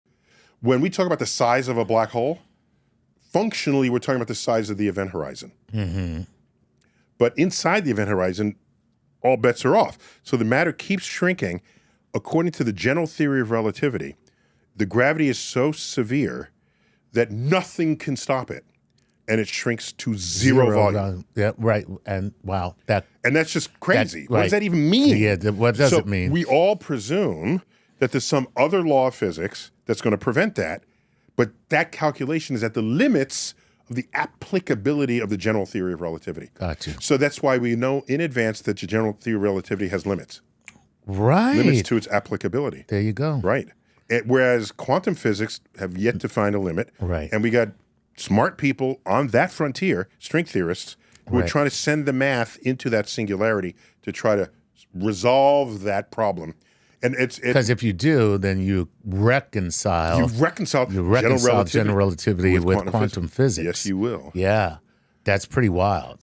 Black Hole Secrets: Event Horizon & Shrinking Matter Explained
Beyond a black hole's event horizon, matter shrinks to zero volume, challenging our understanding of physics. The general theory of relativity hits its limits as quantum physics steps in to reconcile gravity.